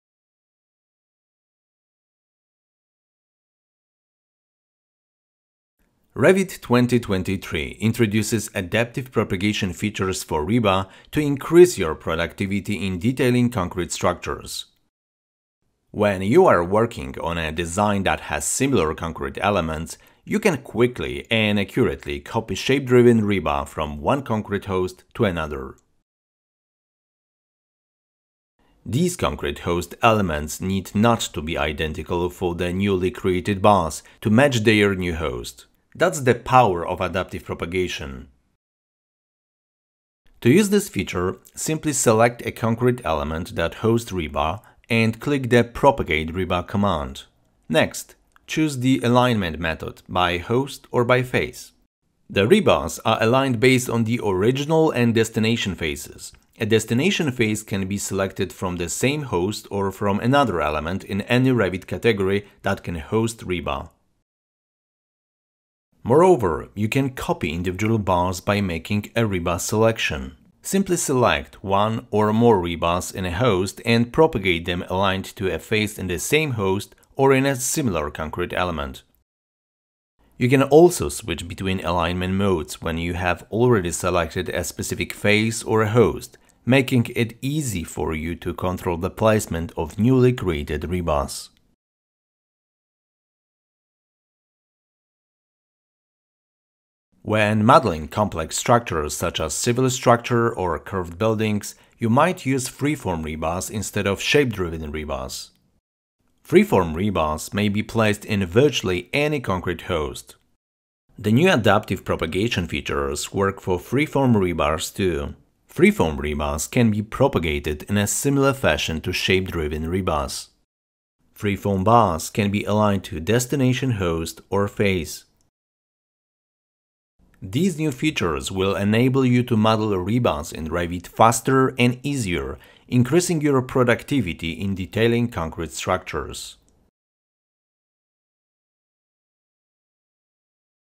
Revit 2023: Adaptive propagation for rebar
Model rebars faster and easier with adaptive propagation